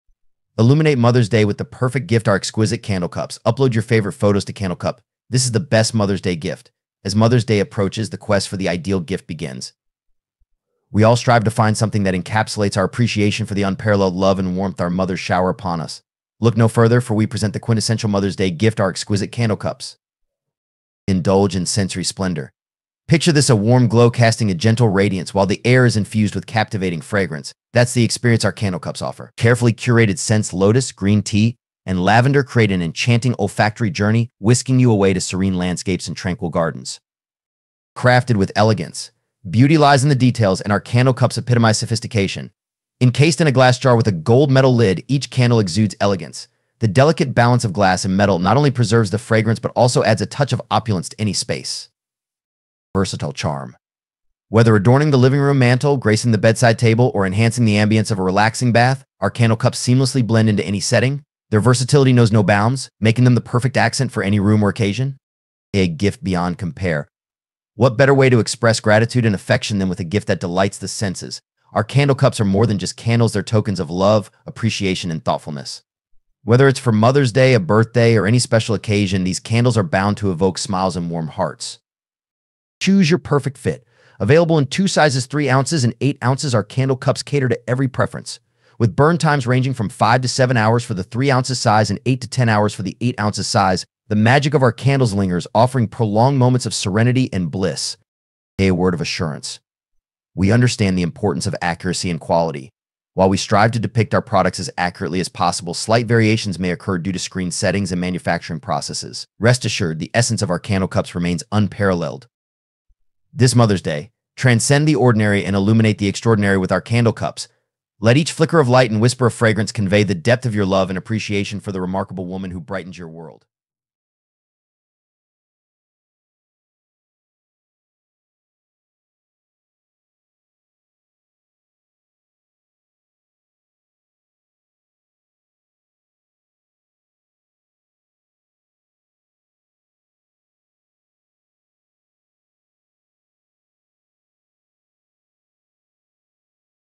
Candle Cups for mother's Day: Illuminate Mother's Day with Our Exquisite Candle Cups
✔️ Similar product links. ⤸

⭐ = Dive into a world of warmth and fragrance with our exquisite Candle Cups, the ultimate Mother's Day gift. Crafted with care and elegance, these candles promise to elevate Mom's special day to new heights of luxury and delight.

𝑽𝒊𝒅𝒆𝒐 𝑬𝒅𝒊𝒕𝒊𝒏𝒈: Filmora Software
𝑬𝒅𝒊𝒕𝒐𝒓: Abdullah As Sayem
𝑽𝒐𝒊𝒄𝒆: Yubair Hossain
𝑺𝒄𝒓𝒊𝒑𝒕: Atiqur Rahman

𝑪𝒐𝒎𝒑𝒖𝒕𝒆𝒓 𝒄𝒐𝒏𝒇𝒊𝒈𝒖𝒓𝒂𝒕𝒊𝒐𝒏.

𝑷𝒓𝒐𝒄𝒆𝒔𝒔𝒐𝒓: Intel(R) Core(TM) i3-6100 CPU @ 3.70GHz 3.70 GHz